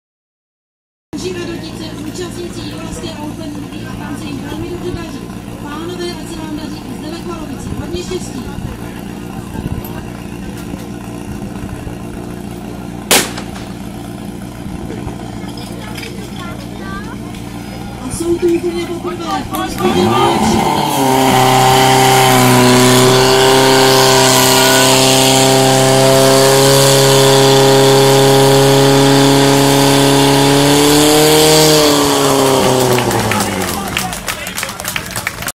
SDH Radotice ZN Liga ve Chvalovicích 2013 první útok (z jiného úhlu)
Zavítali jsme na zkoušku na znojemsko. První útok a čas 17,75 (PP 17,18).